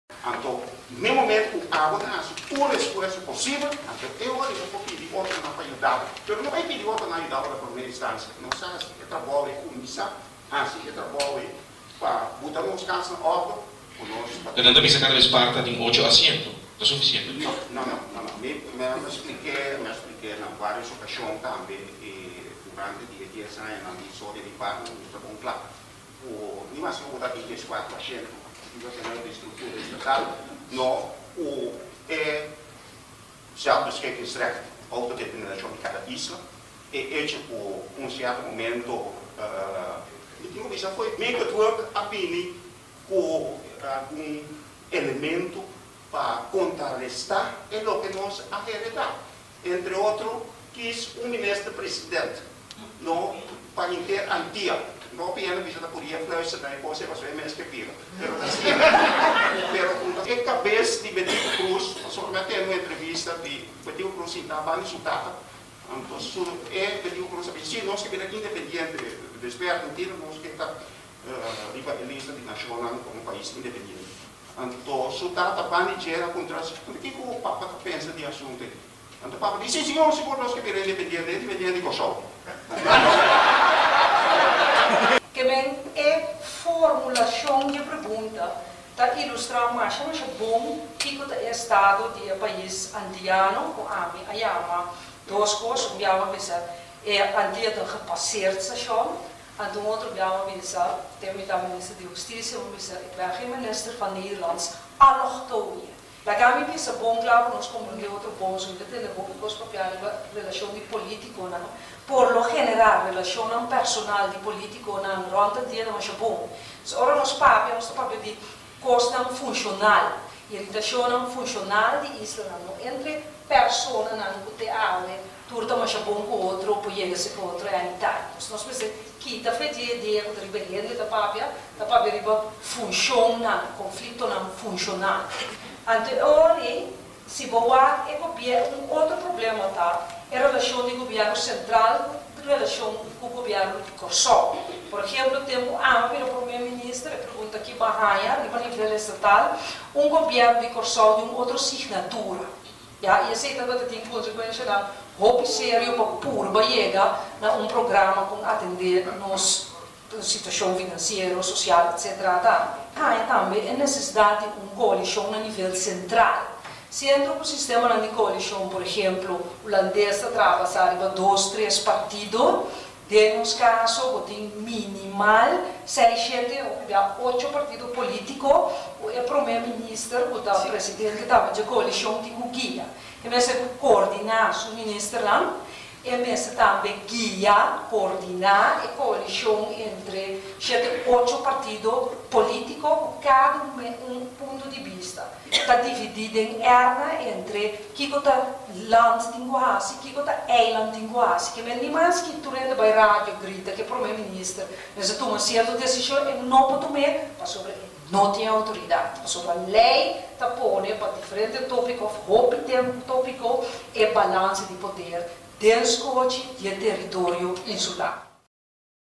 Konferensha eks Prome Minister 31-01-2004 sr. Minguel Pourier, sra. Suzy Camelia-Romer i sr. Etienne Ys.